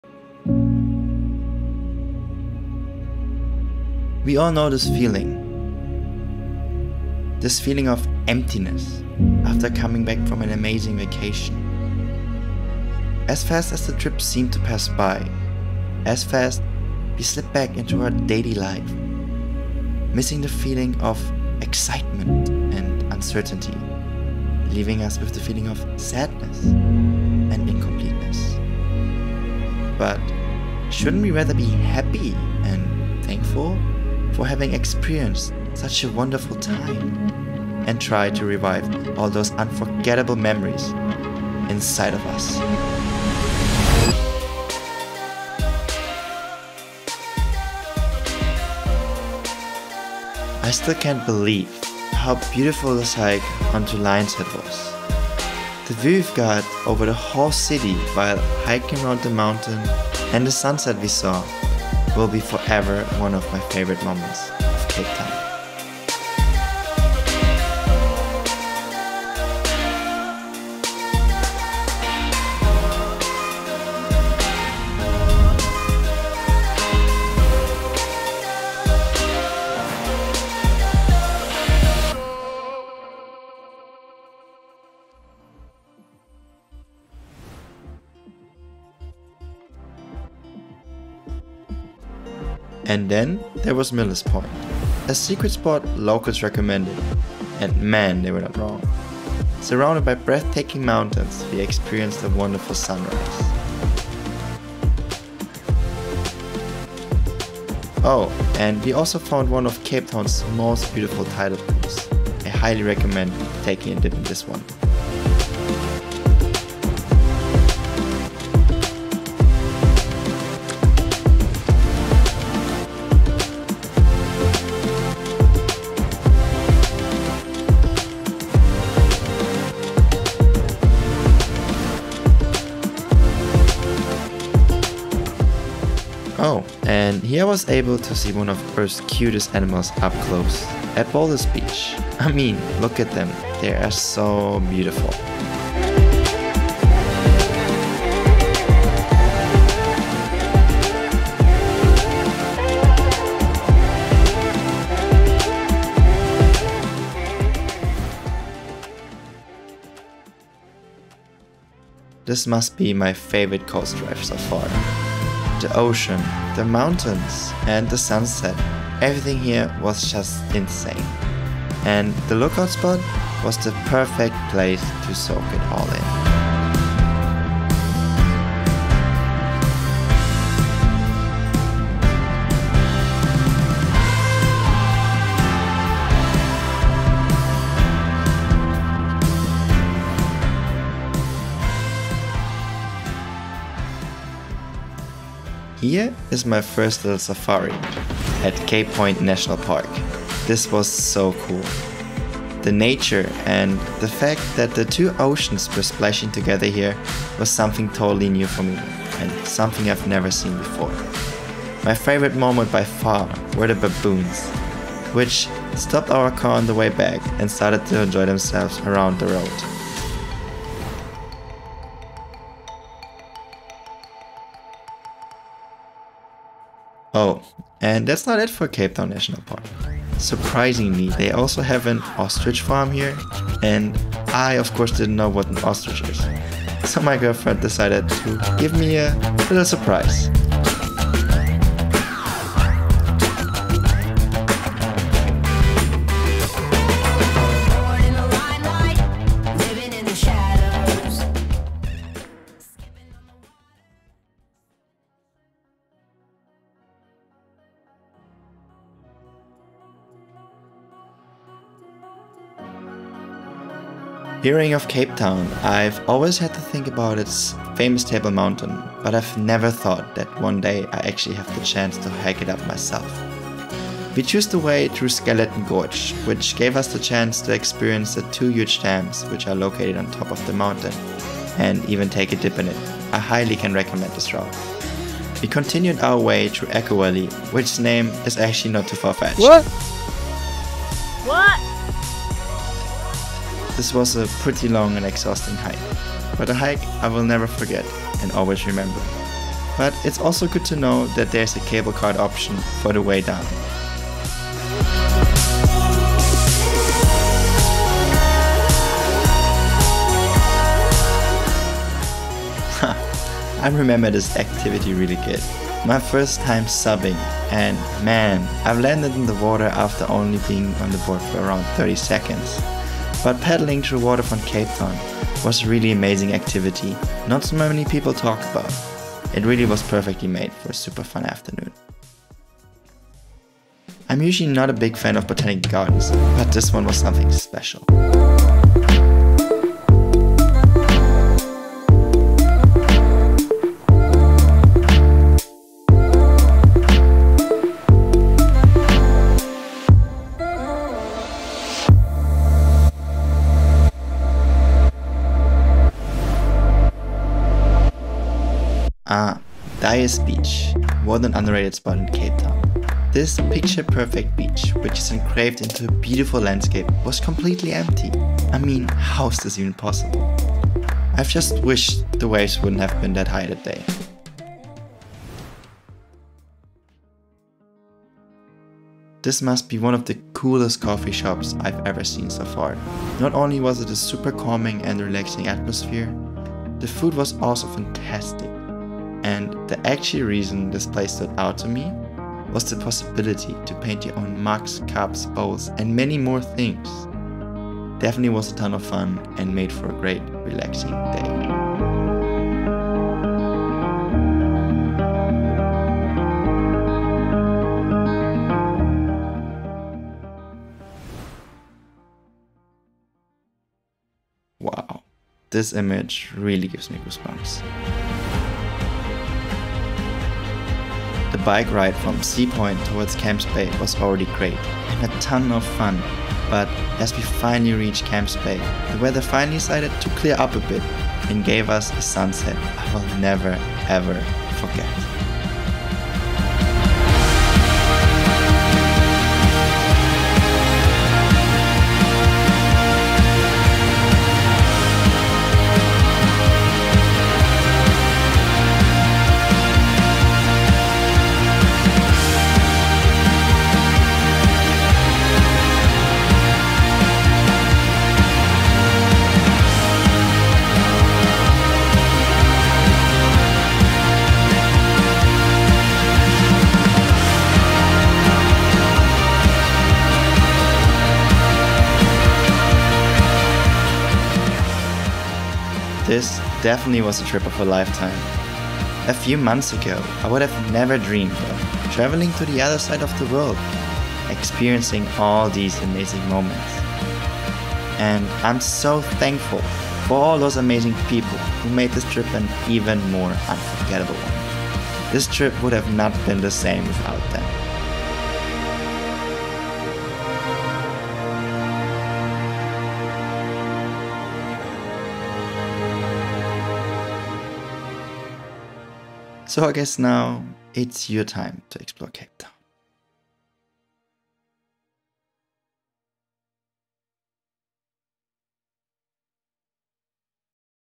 Things To Do in Cape Town | Cinematic Travel Guide 2022
What are the best Things to Do in Cape Town? In today´s Cinematic Travel Guide 2022 I will show you all of the best activities you can do in Cape Town including things like Lions Head, Millers Point, Boulders Beach, Champman´s Peak, Cape Point, Ostrich Farm, Table Mountain, SUPing at Waterfront, Kirstenbosch, Diaz Beach, Clay Cafe, Bike Riding and Camps Bay. With this Cinematic Travel Guide 2022 I want to show you Cape Town in the most beautiful way possible and assist you on choosing the best Things to Do in Cape Town, so your next travel to Cape Town will be an unforgettable one.

Follow for more content:

00:00 Introduction
00:41 Most beautiful hike
01:33 Secret Spot only locals know about
02:17 Meeting the worlds cutest animal
02:46 Magical Coast Drive
03:17 Cape Town´s most beautiful nature
03:50 An unexpected Surprise
04:27 Best hike of Cape Town
05:12 Unknow fun activity
05:36 A special botanical garden
06:06 Best Beach of Cape Town
06:23 Cape Town´s special coffee shop
06:55 My favorite memory of Cape Town
07:55 This is for you